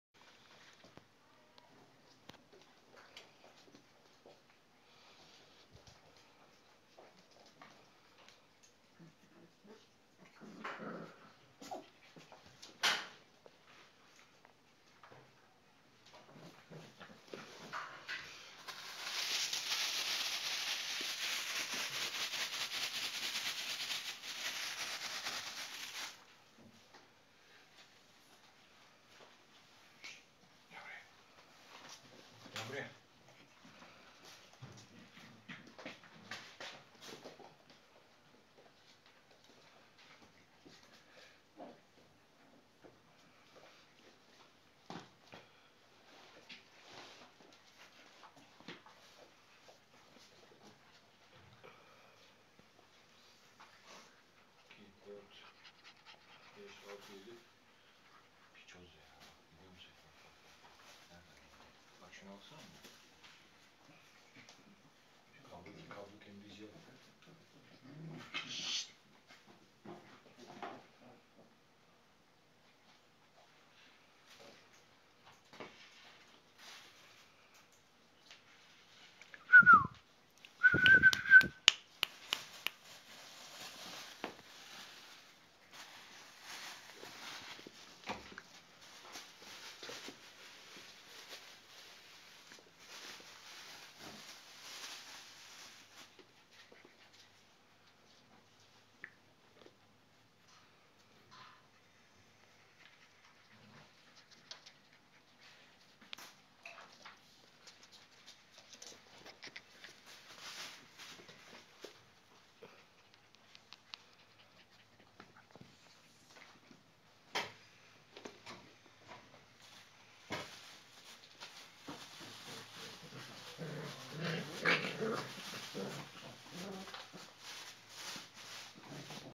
CRAZY ROTTWEILER PUPPIES
Cromo vom Sittard X Asha vom Goldenhorn puppies
34 days old